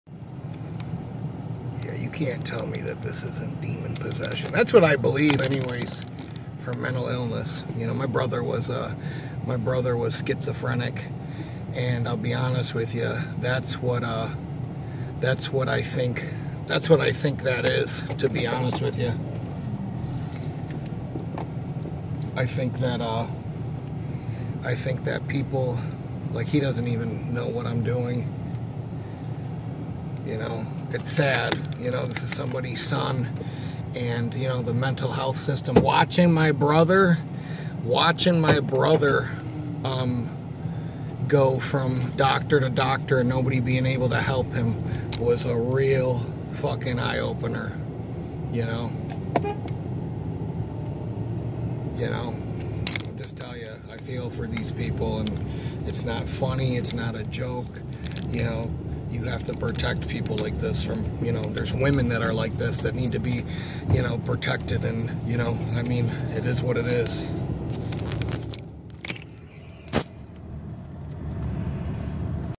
There is Demons inside these ppl
my brother suffered w schizophrenia most of his life...